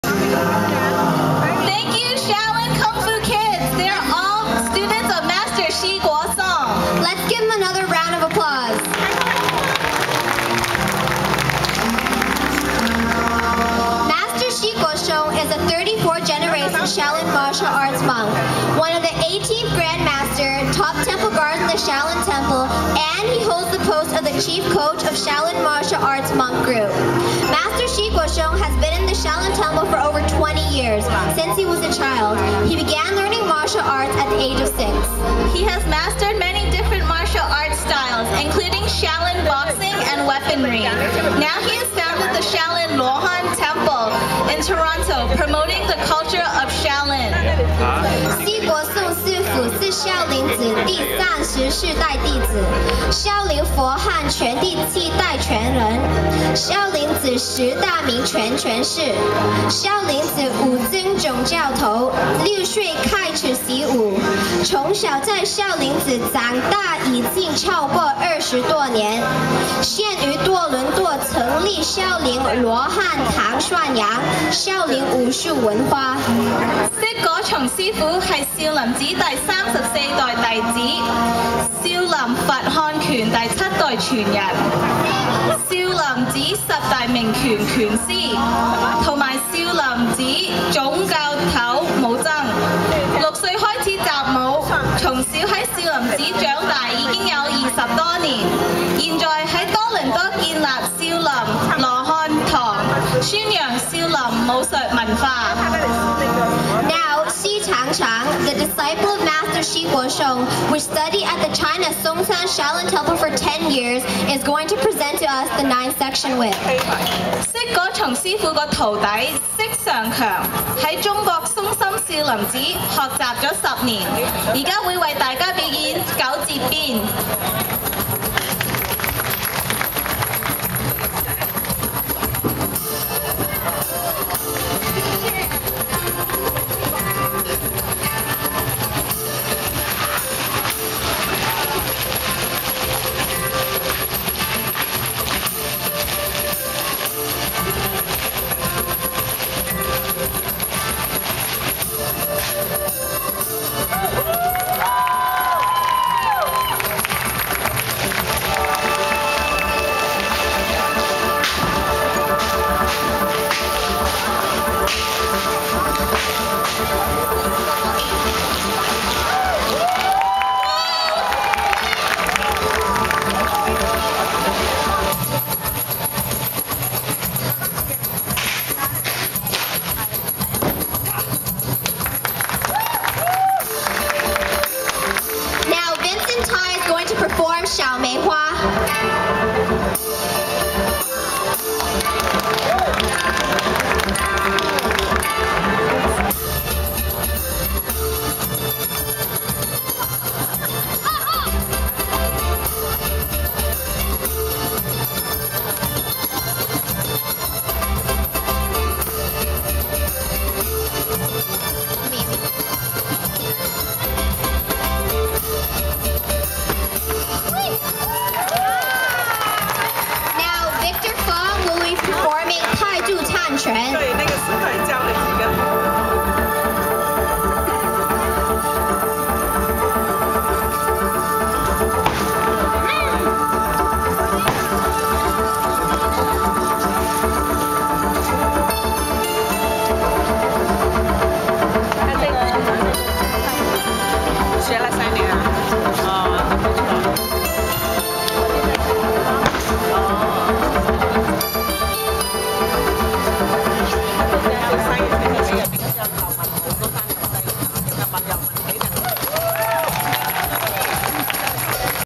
Taste Of Asia 2010 Shaolin Kung Fu Performance Part 3
Shaolin Kung Fu performance during Taste Of Asia 2010 by Master Shi Guo Song, his disciple and students.  Master Shi Guo Song is a 34th generation Shaolin monk currently teaching kung fu and chi kung (qigong) at Markham Chinese Cultural Centre in Market Village in Markham Ontario, Canada. There is a good video clip of Master Shi Guo Song from the Discovery Channel.